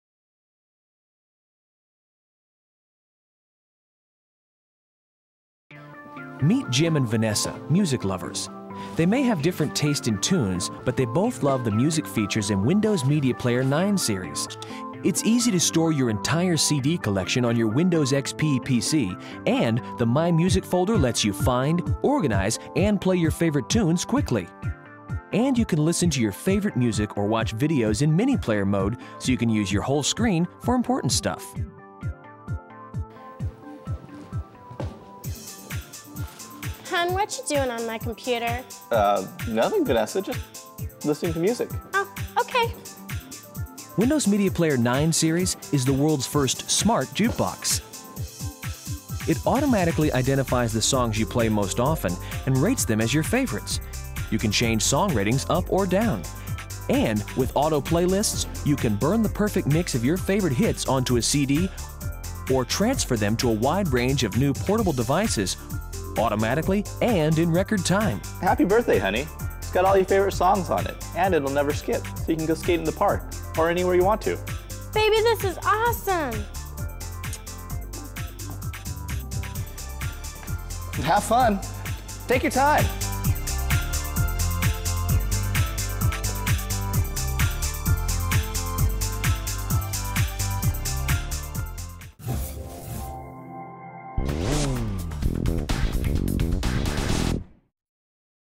Meet Jim and Vanessa, Digital Music with Windows Media Player
Old MS promo video from the Windows XP era.  Do Amazing Things with Windows XP, YES You Can!